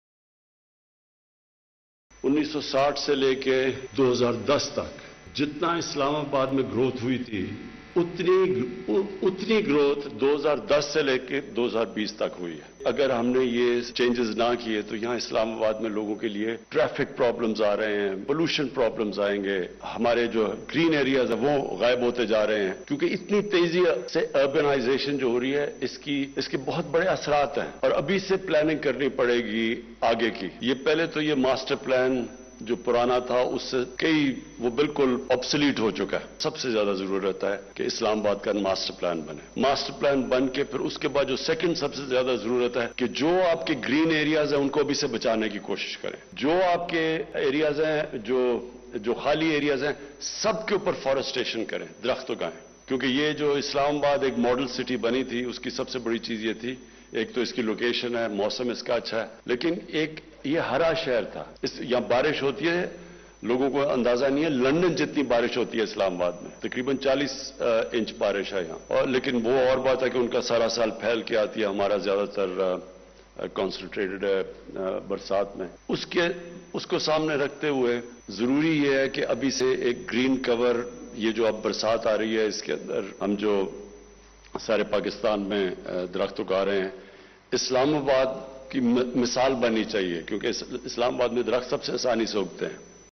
Prime minister lauded the performance of CDA
ISLAMABAD, July 14 (APP): Prime Minister Imran Khan emphasized on making Islamabad a model for rest of the country in terms of plantation. He expressed satisfaction that CDA had revamped several parks to provide citizens and families a place for leisure time.
The prime minister lauded the performance of CDA for generating revenues worth Rs 136 billion compared with the deficit of Rs 5 billion at the time his government took to power.